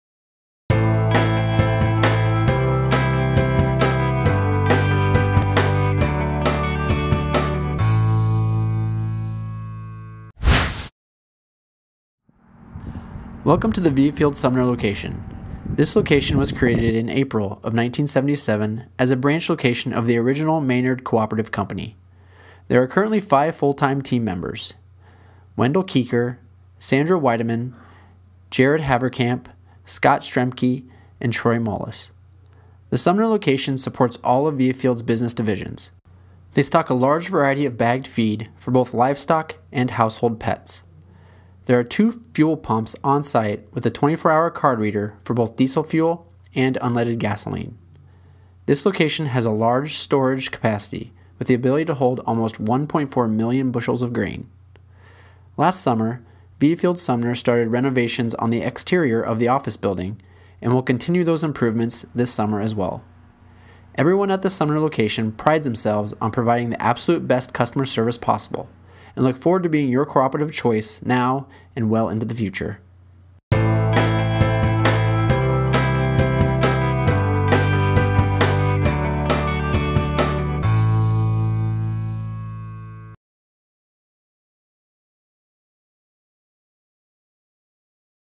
Sumner Location Spotlight
The Viafield Sumner location was created in 1977 as a branch for the original Maynard Cooperative Company prior to the Viafield unification. In this location spotlight, we give you a first hand tour of the facility and discuss some of the features that make this location so important to the Sumner community and to Viafield.